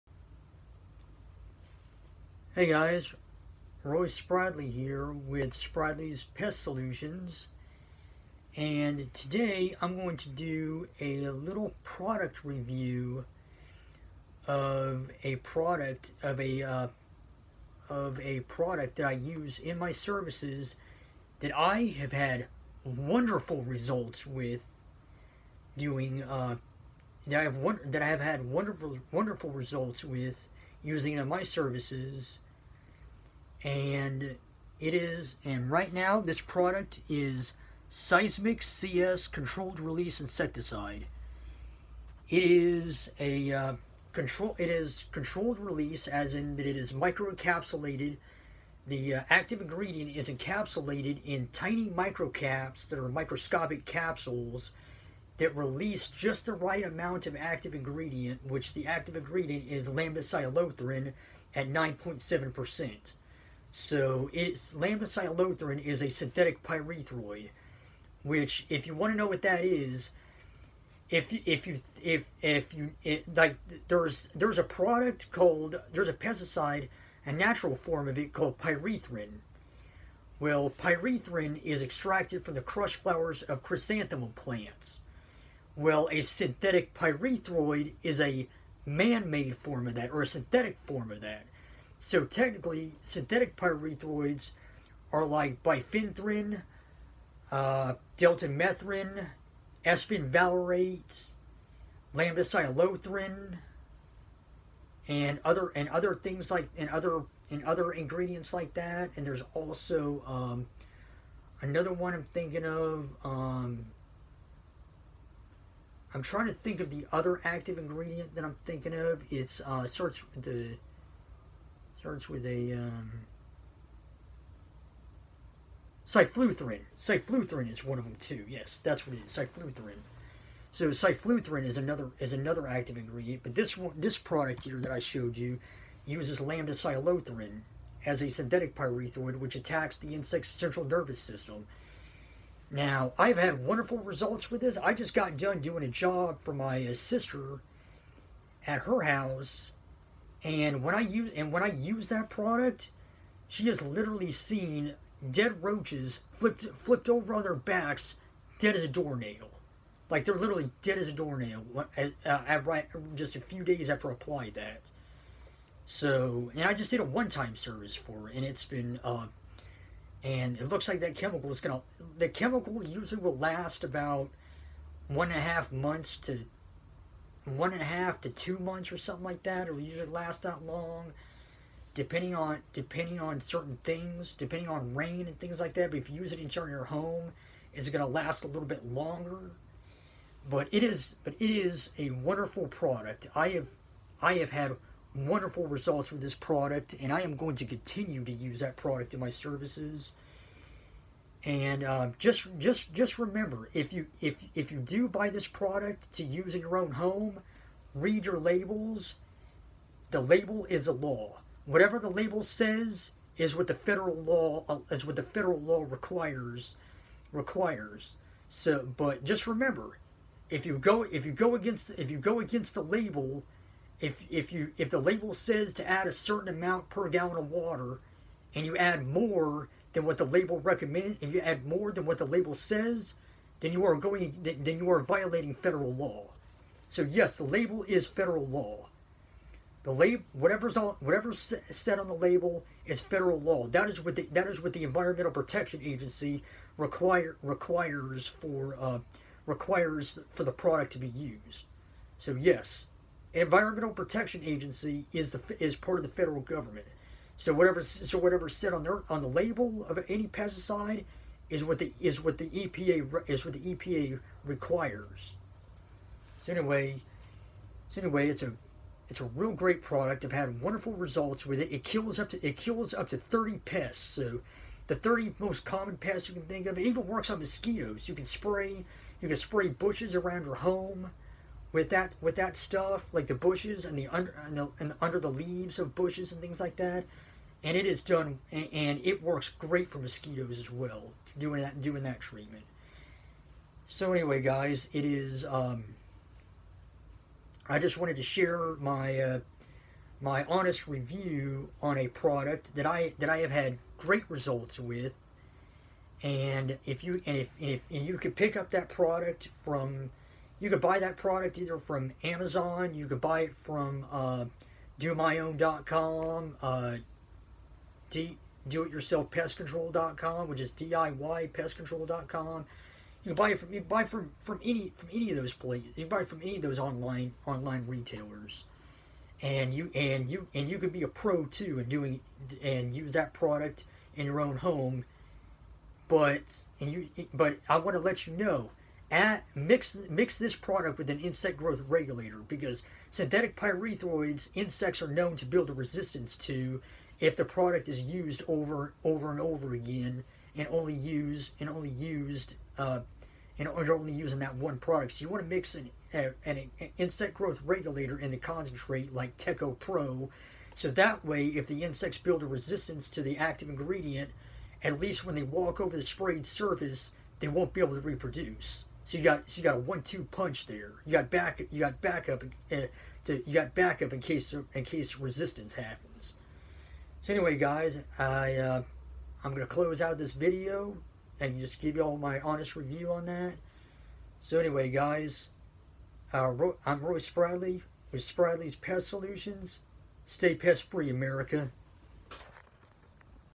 My review of Cyzmic CS Controlled Release Insecticide
My review of an insecticide that I use in my services that has brought wonderful results for my company as well as my clients